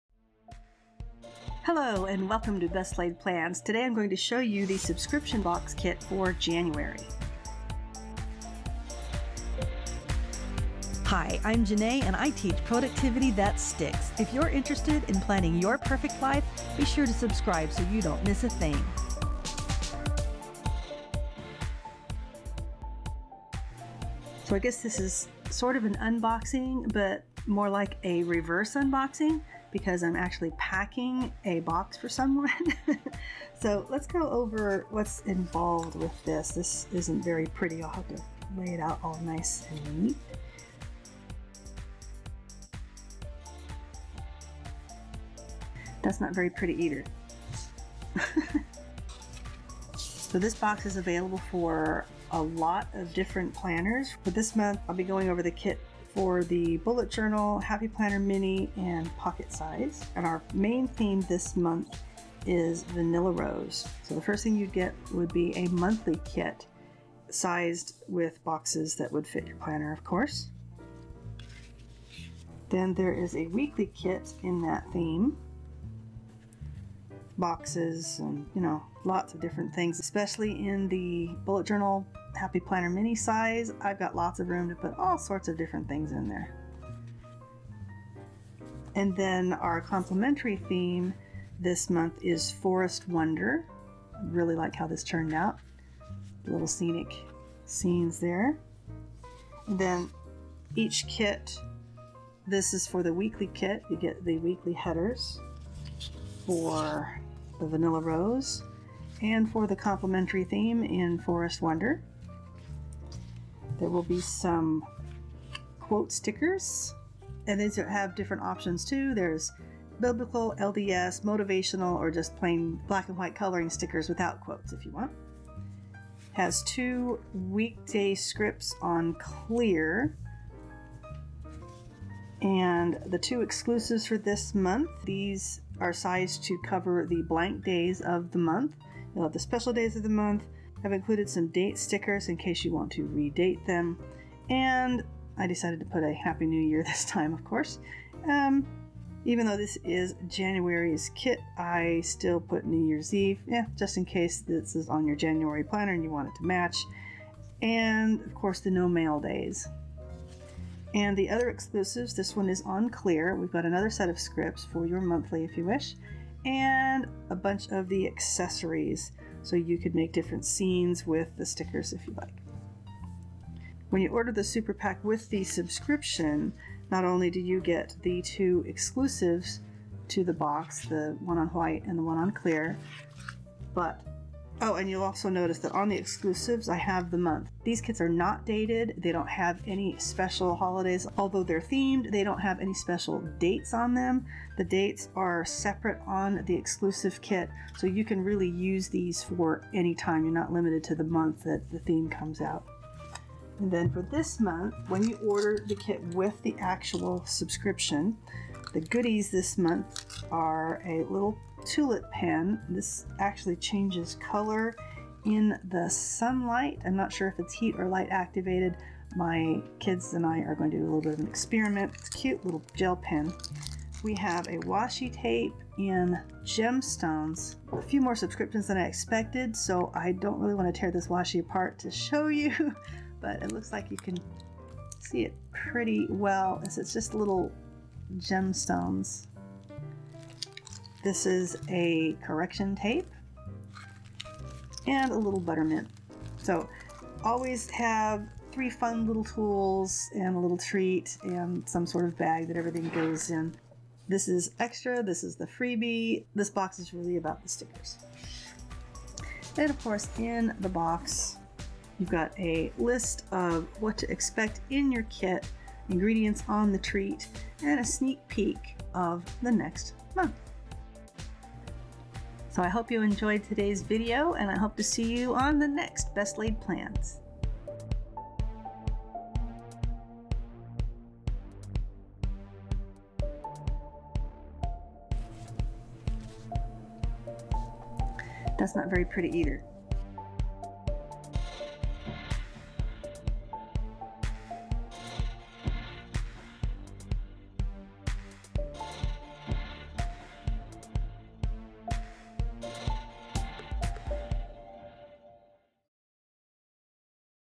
Plans That Stick Planner Sticker New Releases Vanilla Rose, Forest Wonder - Leafy Treetops EC HP BJ
Today we're going through the new themes for November 2019, which are in the subscription box shipped in November, including exclusives for January 2020.  Vanilla Rose and Forest Wonder are shown in the HP Mini / Bullet Journal / Pocket size format, along with the exclusive stickers and tools that came with the subscription.  The stickers are available separately, and all together as a SuperPak.  The stickers and subscription are available in either vertical or horizontal for Happy Planner Classic and Mini, Erin Condren, BuJo, Pocket, and In The Leafy Treetops.  Hobonichi and PP Weeks available when requested.

No products in this video were compensated promotions.
I either endorse these products or create them myself, so if you buy from me, you'll help feed my herd of kitties.  Oh, and my kids.